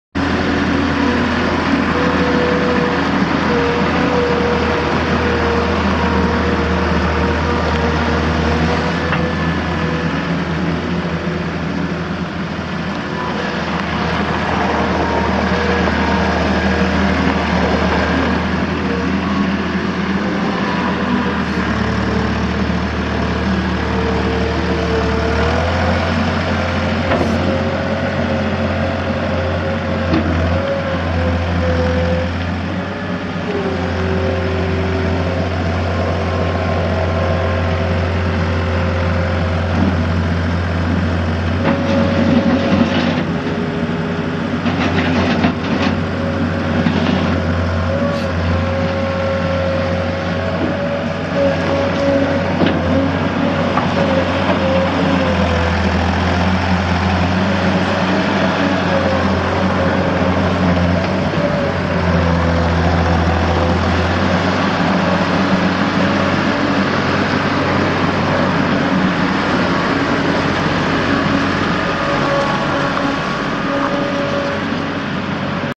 2000 John Deere 260 skid steer for sale at auction | bidding closes January 31, 2019
2000 John Deere 260 skid steer
1,652 hours on meter
John Deere three cylinder diesel engine
Hydrostatic transmission
Open station/canopy
72"W bucket
Hydraulic quick coupler
Auxiliary hydraulics
Counter weights
14-17.5 tires
Non-operational oil pressure gauge
Serial 260466
It's located in Stigler, OK, and we're selling it to the highest bidder regardless of price.